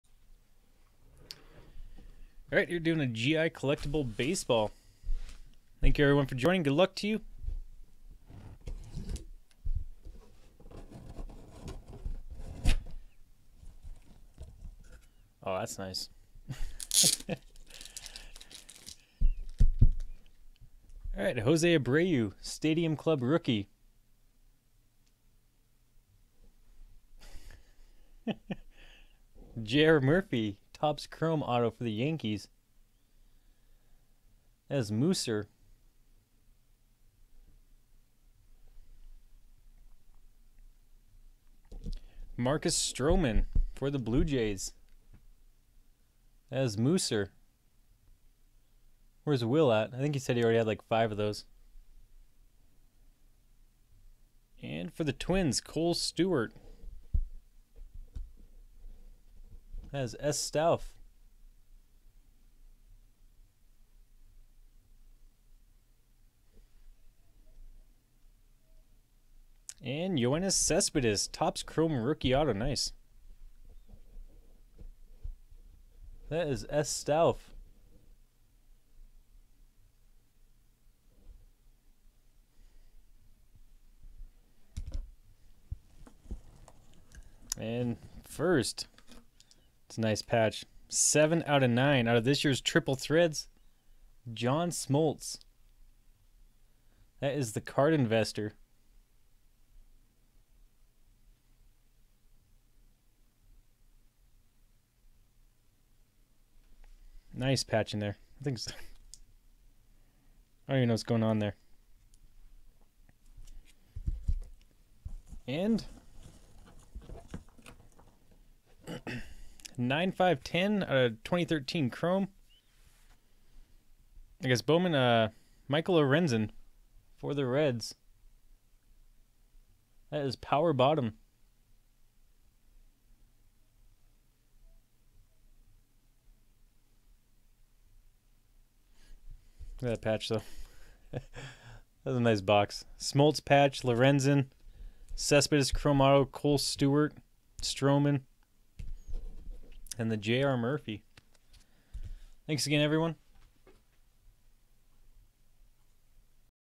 1 Box GI Collection Baseball Wave 3 #3
Thanks for joining the break everyone, and good luck!

sstauff al west
TheCardInvestor nl east
PowerBottom nl central
sstauff al central
sstauff nl west
mooser1974 al east